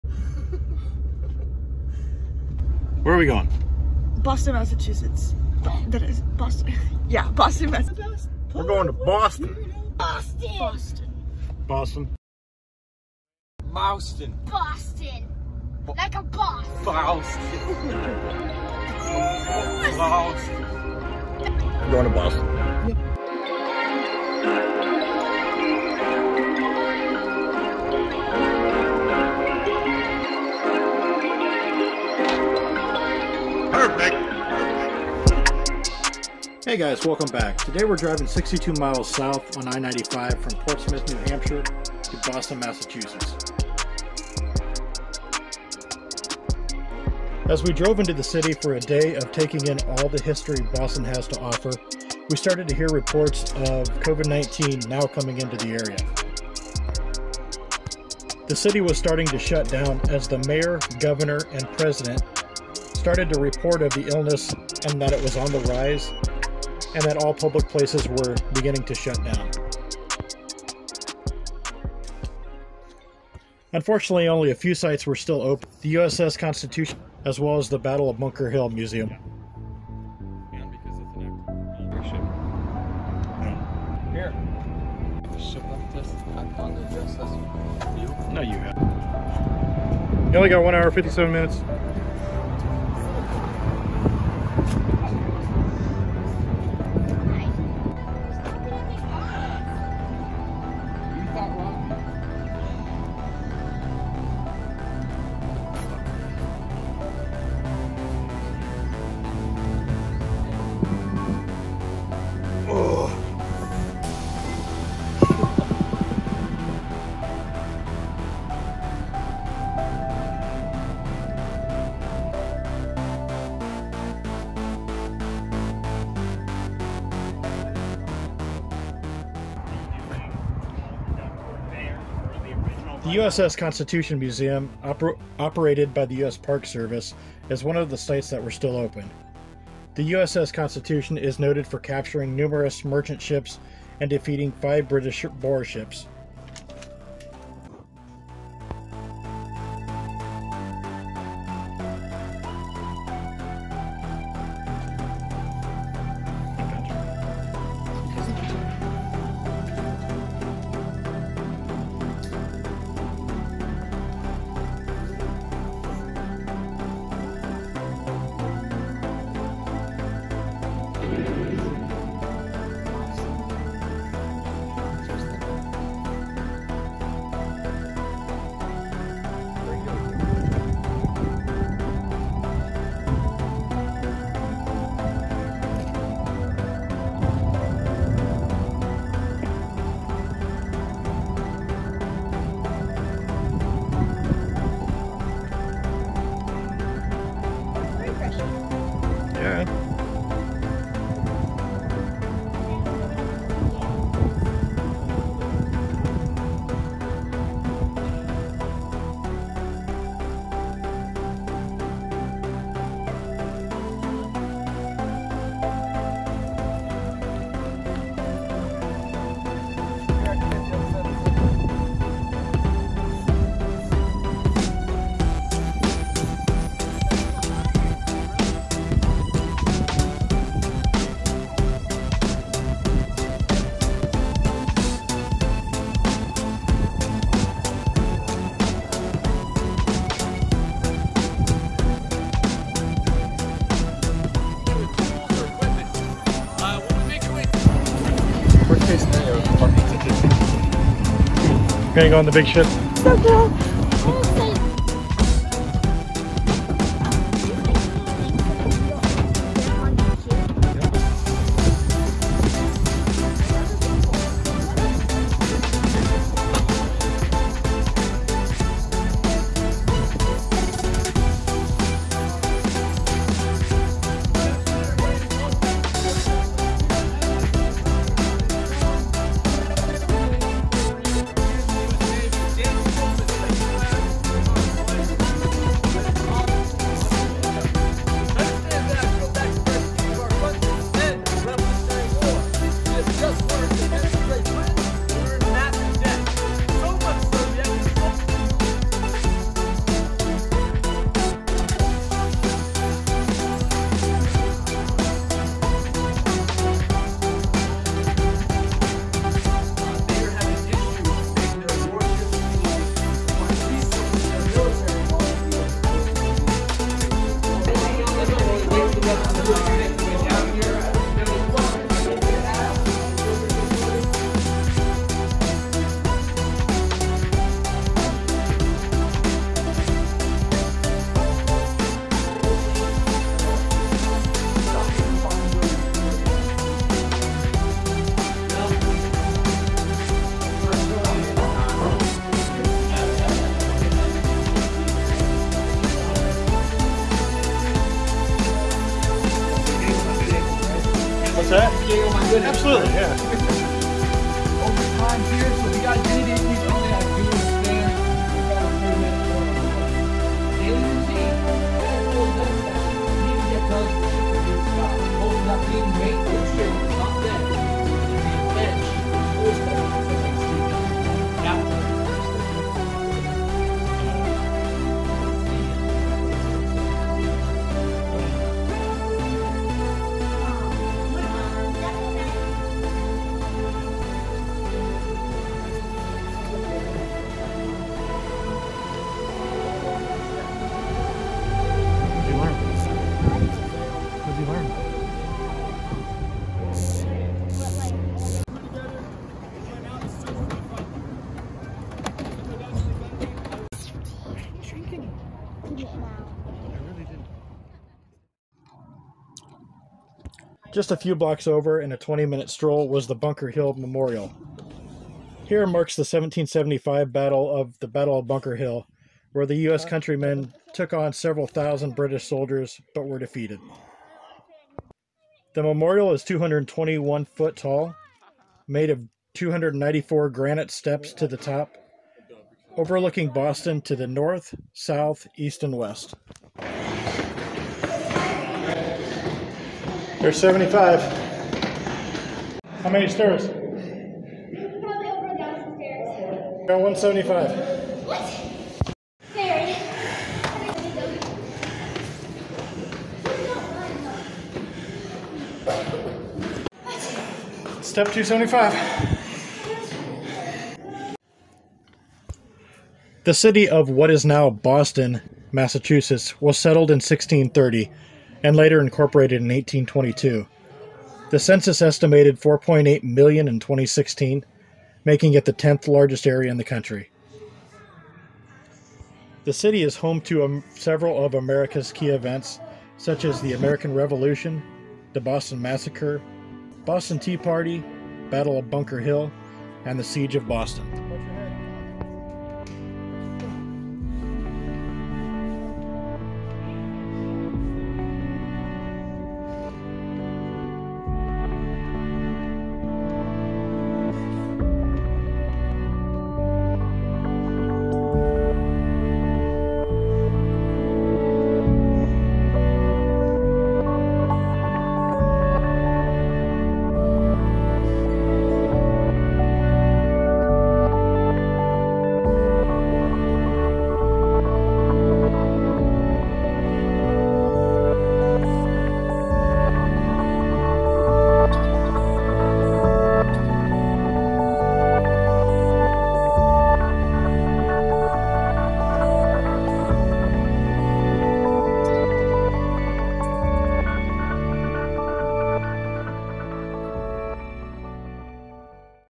Pre Covid 19 Boston Sight Seeing
We've been stuck at home bored, bored, bored.  Well, not really.  We've been doing a lot of moving around.  We are though, running low on some footage, so we thought we'de finally finish this video we took, just days before the Covid-19 epidemic blem up in Boston.  We hope you are all safe at home, healthy.  Or if you are out fighting this fight, doing good.  We hope you stay safe and well.  We are all in this together and we'll get through this together.